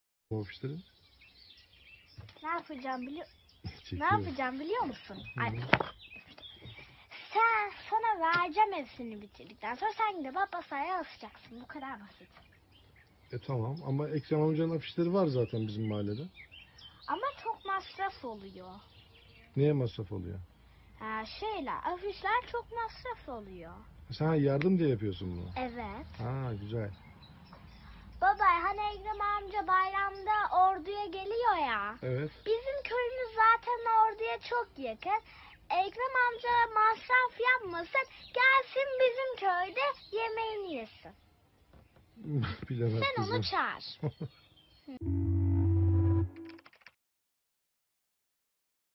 Küçük kızdan İmamoğlu’na çağrı - Masraf yapma yemeğini bizde ye
Ordu'da bir baba ile kızı arasında geçen Ekrem İmamoğlu diyaloğu izleyenleri gülümsetti. Ordu'nun bir köyünde yaşadıkları anlaşılan ailenin küçük kızı "Bizim evimiz Ordu'ya çok yakın. Ekrem amca masraf yapmasın gelsin bizim köyde yemeğini yesin" dedi.

Haber Kaynak ;